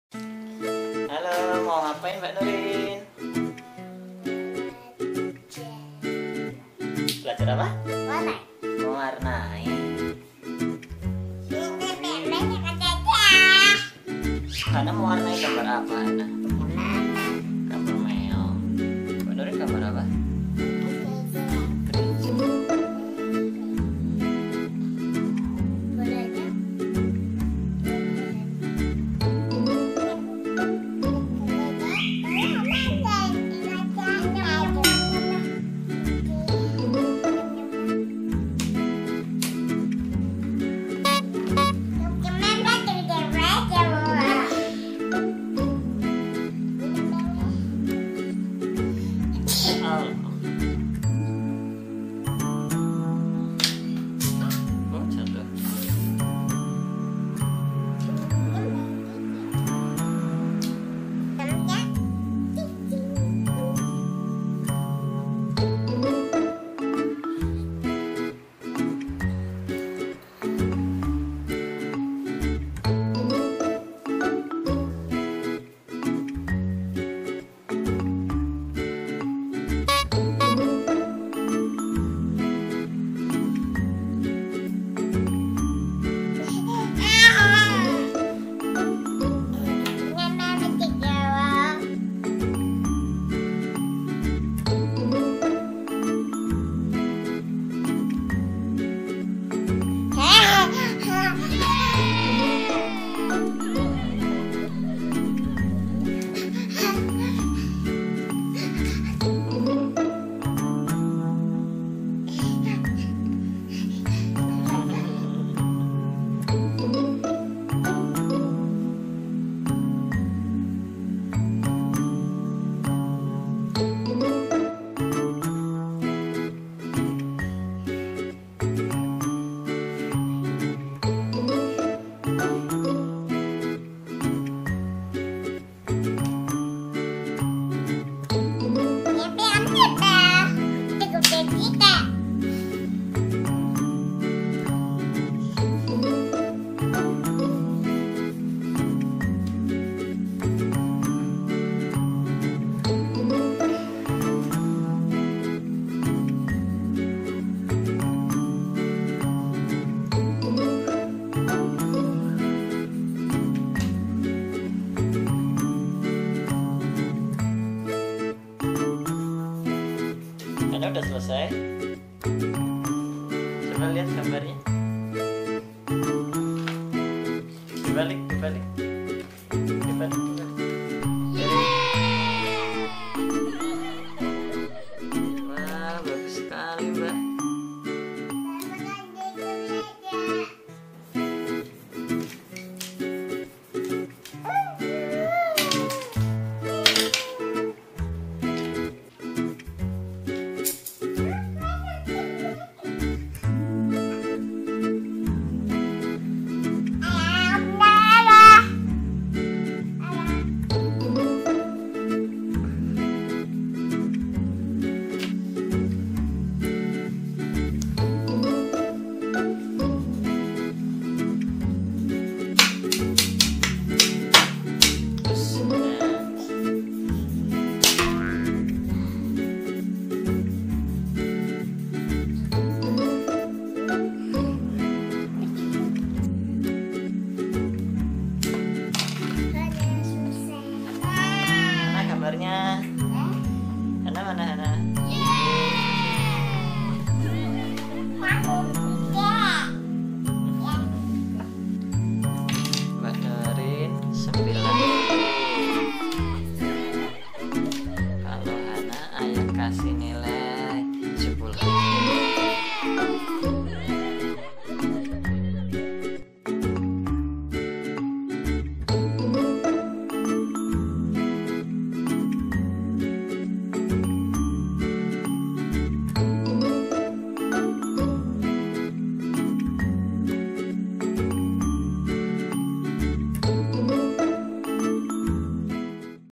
Anak Kecil Belajar Mewarnai Gambar ❤ Kids Coloring Fun
Nurin dan Hana berlatih mewarnai gambar-gambar lucu. Melukis, menggambar dan mewarnai gambar merupakan kegiatan yang selalu disukai oleh anak-anak kecil terutama anak balita.